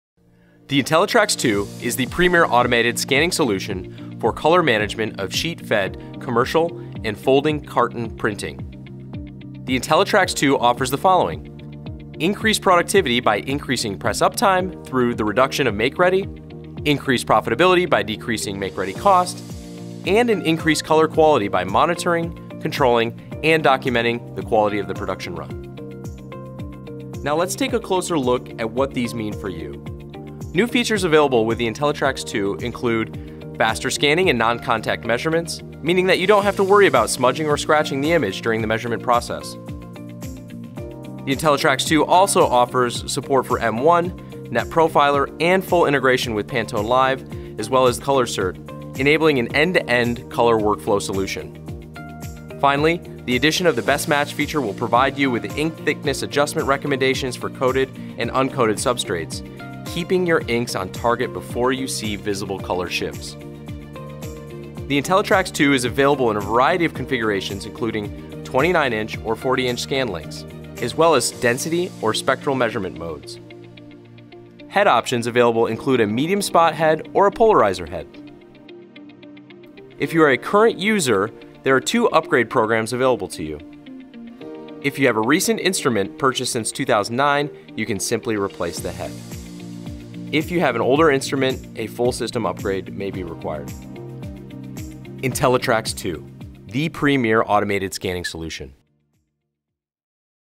IntelliTrax2 - Ultra Fast Automated Press-Side Color Control
IntelliTrax2 is the ideal color management solution for high-end, high-speed printing/converting operations.